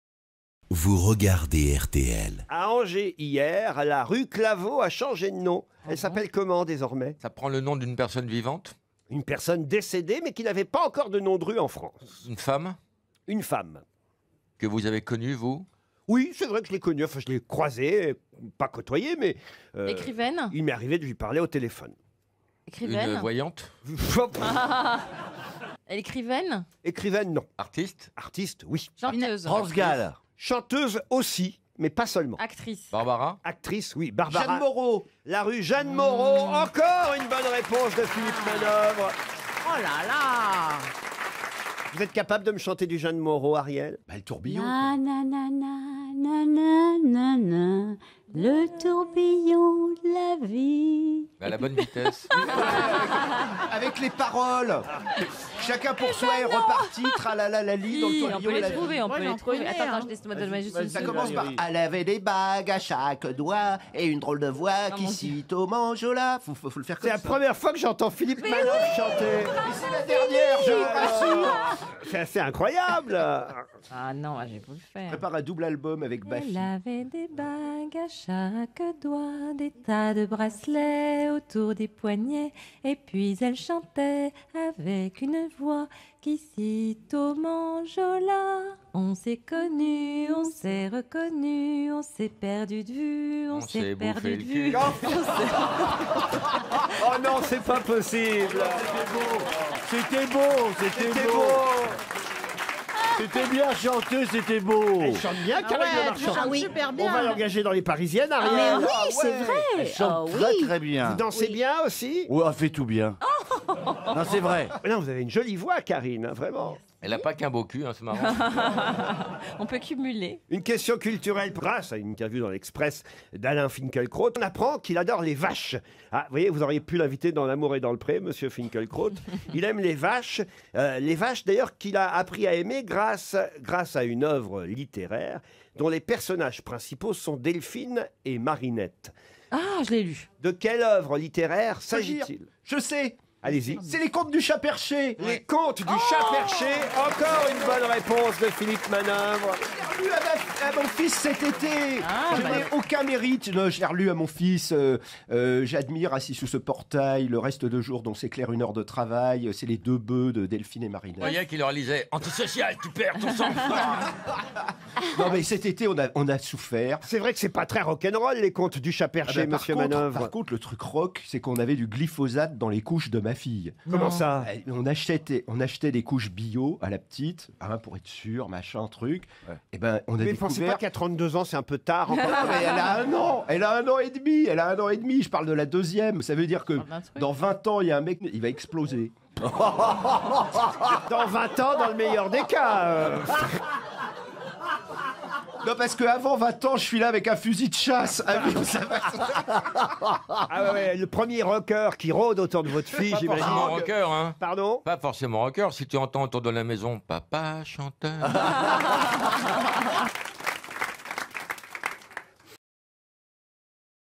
Philippe Manœuvre chante Jeanne Moreau
Le chroniqueur du rock surprend le public en reprenant Arielle Dombasle en train d'interpréter un succès de l'actrice disparue l'an dernier. Et Karine le Marchand prend même le relai en chantant à son tour...